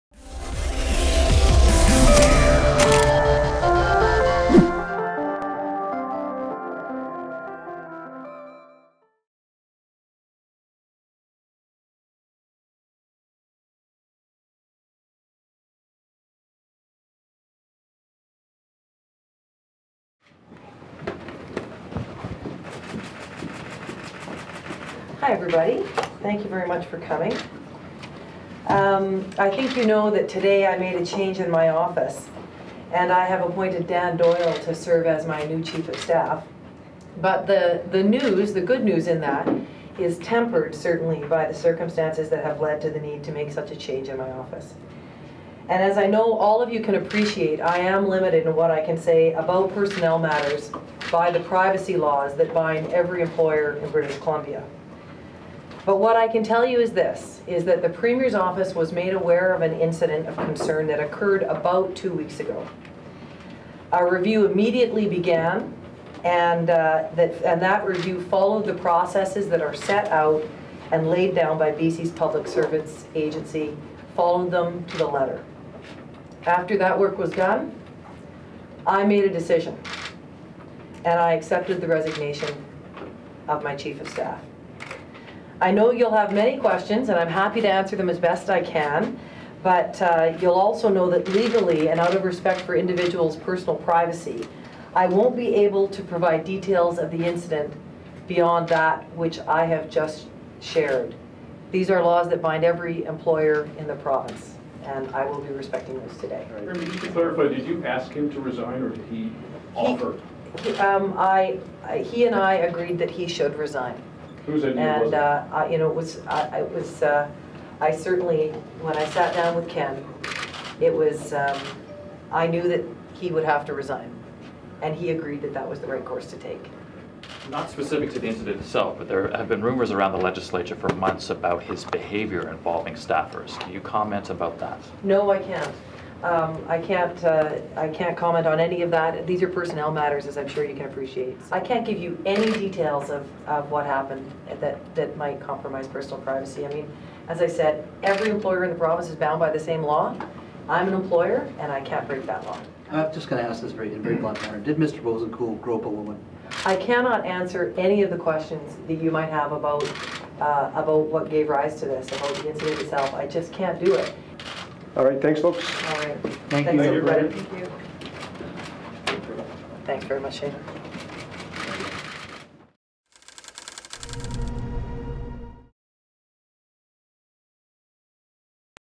B.C. Premier Christy Clark speaks to media following the the sudden resignation of Ken Boessenkool
Premier Christy Clark’s Chief of Staff Ken Boessenkool resigned Monday following an internal investigation into an unspecified incident.
He was replaced immediately by Dan Doyle, a career public servant and former B.C. Hydro chairman.